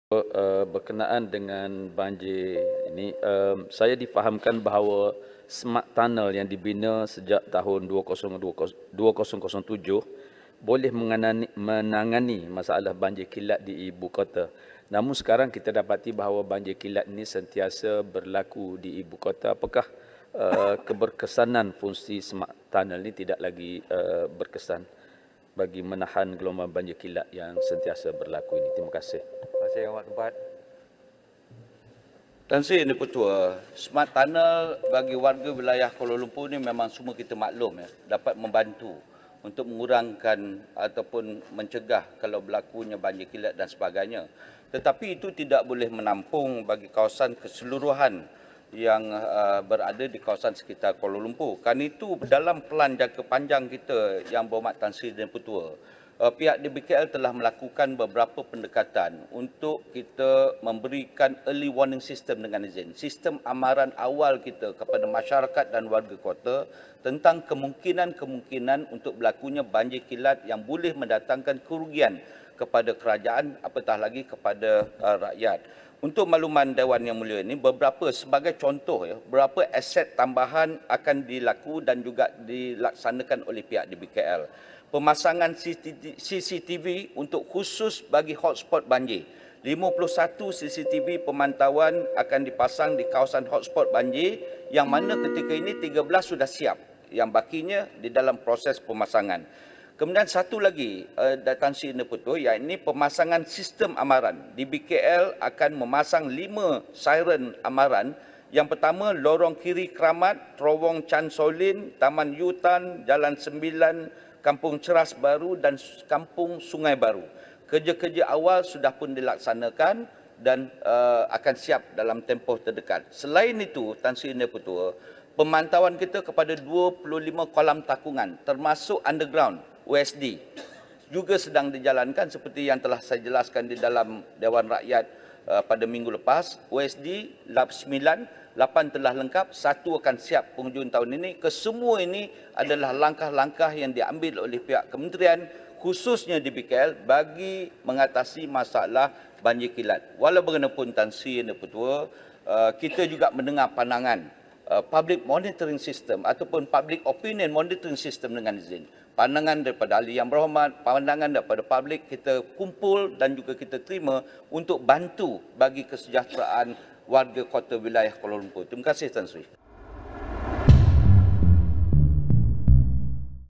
Five flood warning sirens to be installed in KL
Sirens will be installed in the federal capital to provide early warning to city dwellers on the possibility of flash floods, said Deputy Federal Territories Minister Jalaluddin Alias.

He said preliminary work had started for the installation of the siren at Lorong Kiri Keramat 15, Chan Sow Lin Tunnel, Taman U-Thant, Jalan Sembilan in Kg Cheras Baru and Sg Kayu Ara.